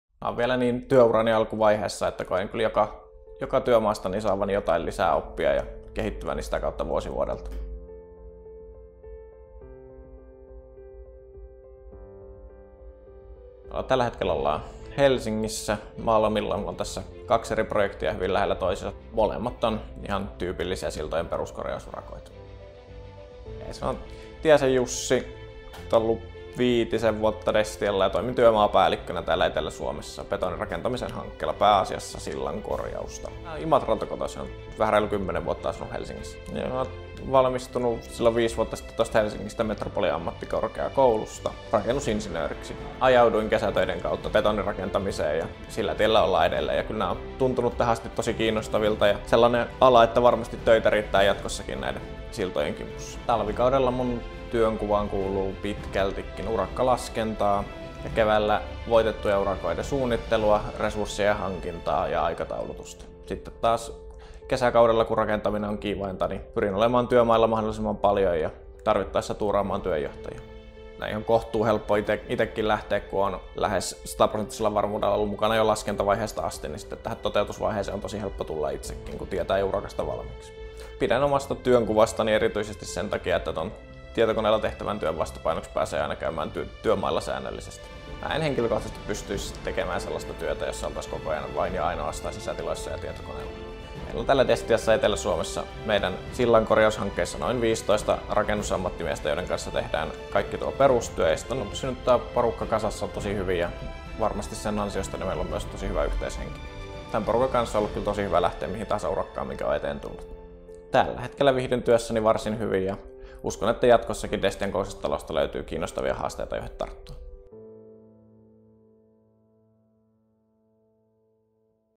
Tositoimissa Jussi Tiainen
Jussi Tiainen työskentelee Destiassa työmaapäällikkönä pääasiassa betonirakentamisen parissa. Toimisto- ja tietokonetyön vastapainoksi Jussi nauttii siitä, että pääsee töihin myös työmaille.